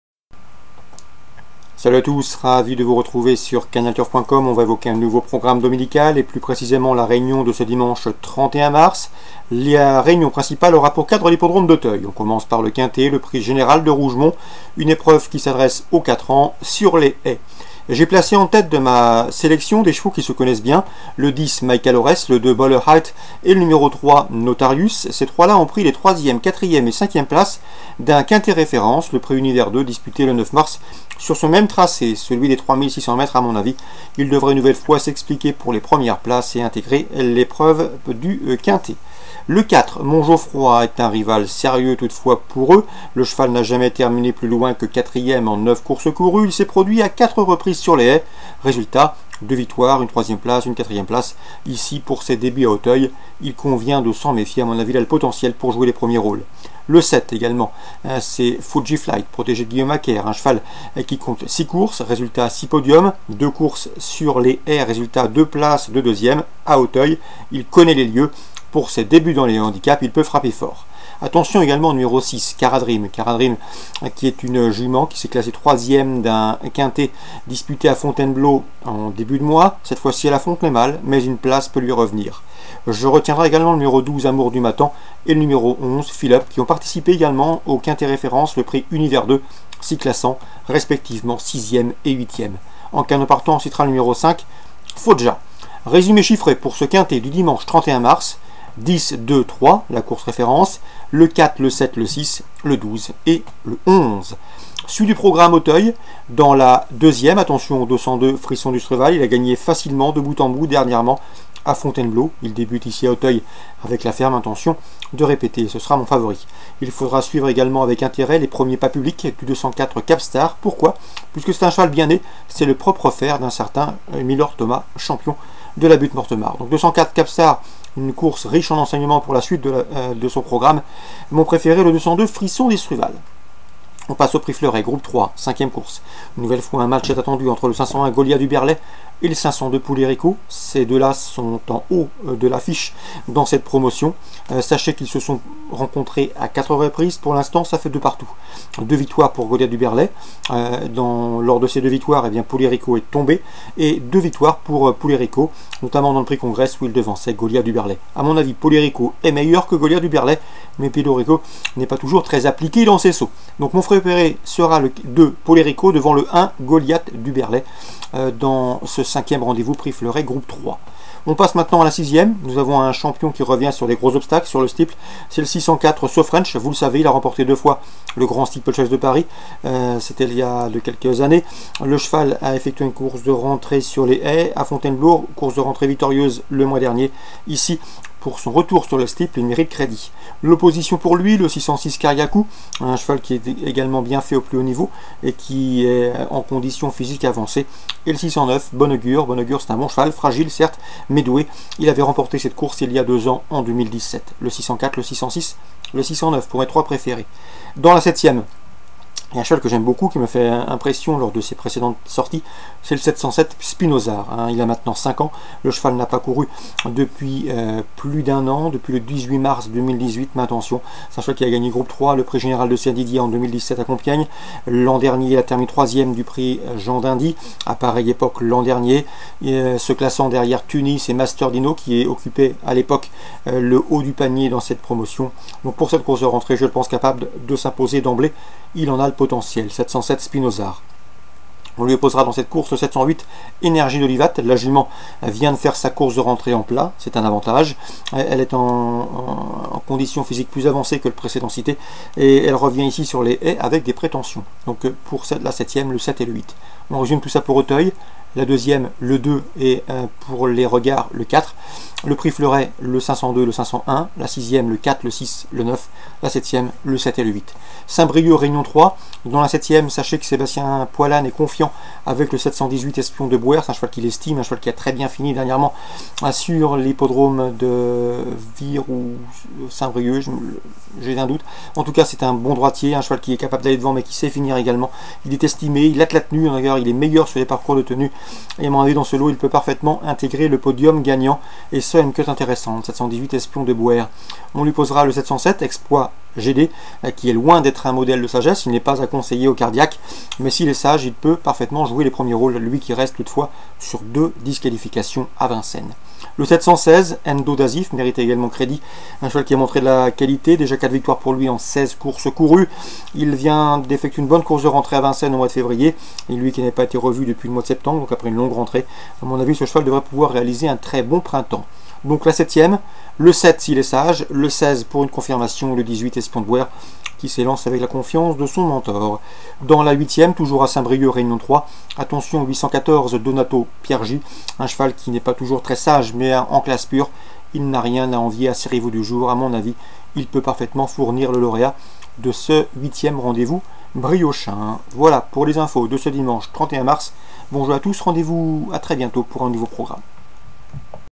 Pronostic du Quinté+ du Dimanche 31 Mars 2019 et des courses PMU
Les pronostics vidéo du Quinté PRIX GENERAL DE ROUGEMONT et des courses PMU du Dimanche 31 mars 2019 par les journalistes de Canalturf

Quinté+ PMU PRIX GENERAL DE ROUGEMONT
15:15 - Réunion 1 Course 3 - AUTEUIL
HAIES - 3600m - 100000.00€